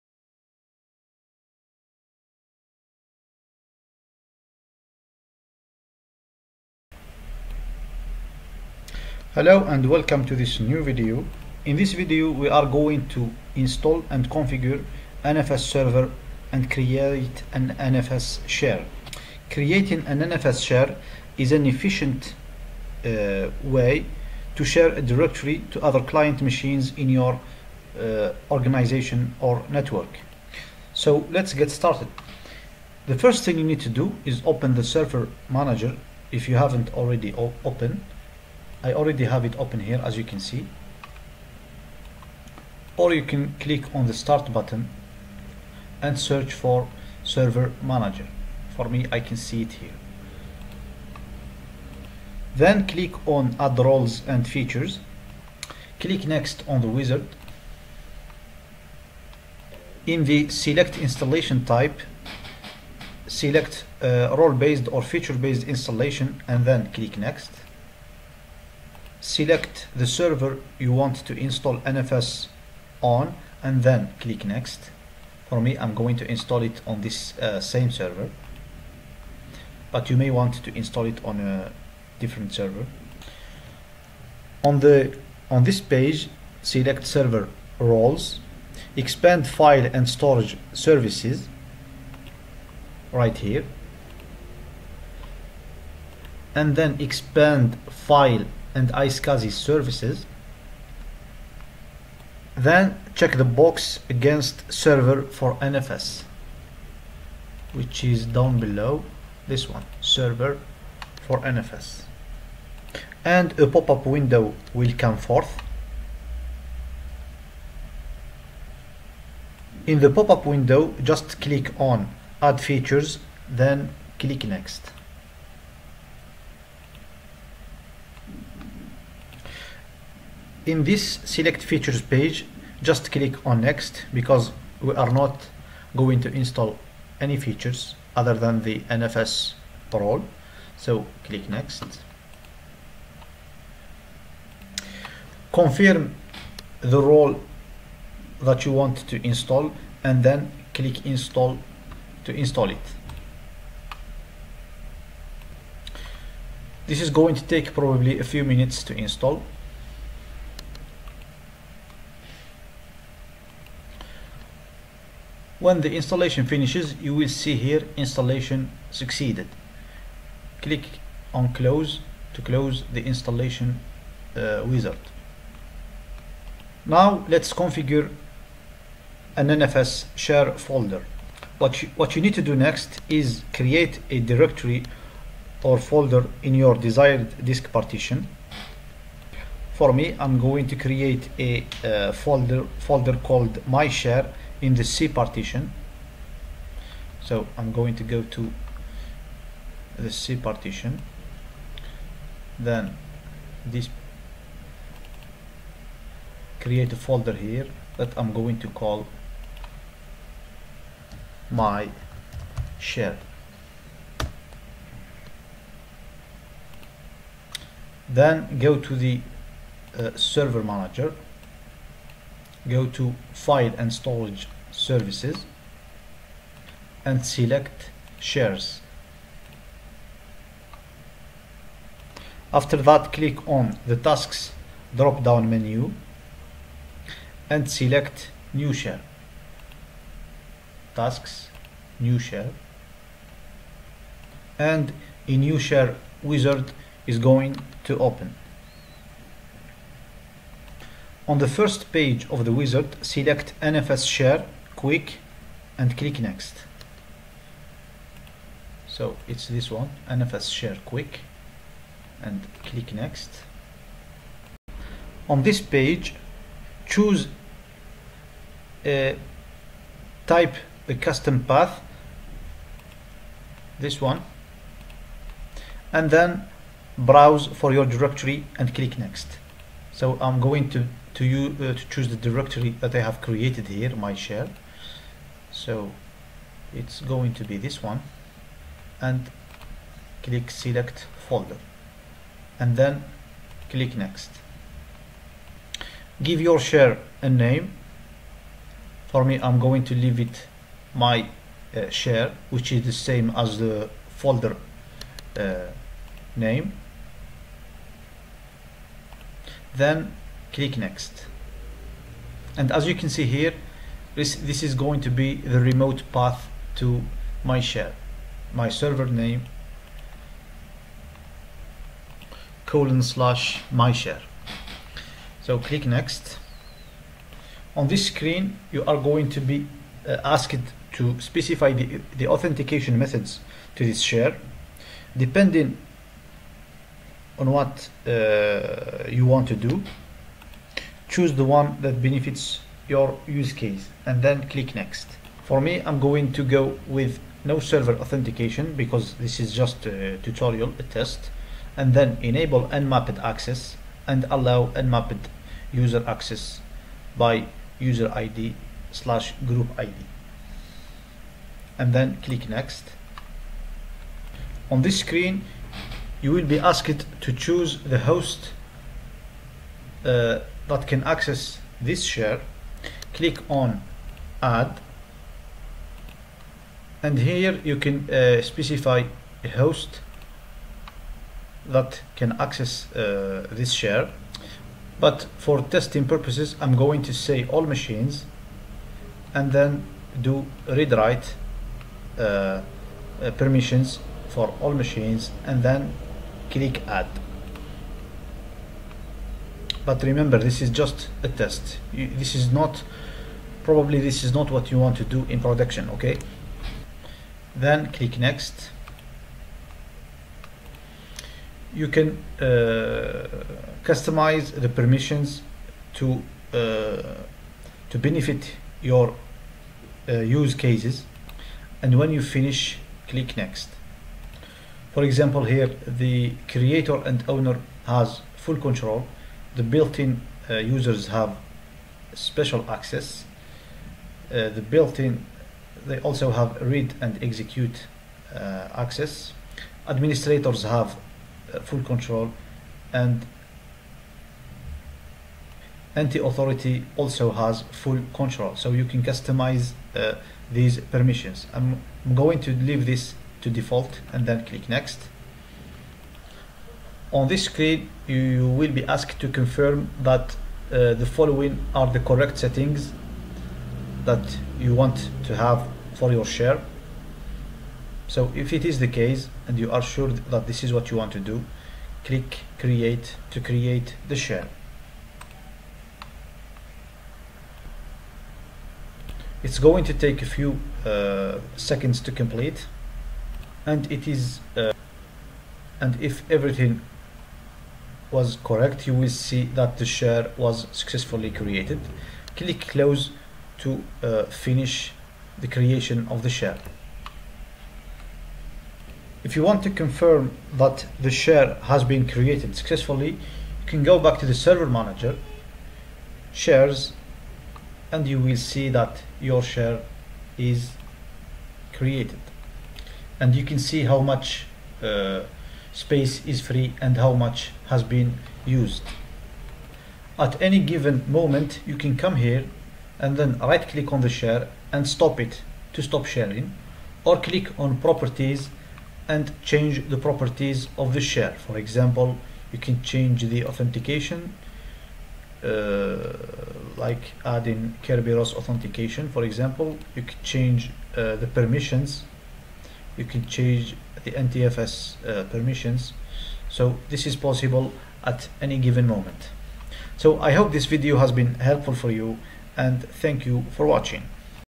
How to Configure NFS Server on Windows Server 2019
In this video, we are going to install and configure NFS Server and create NFS Share.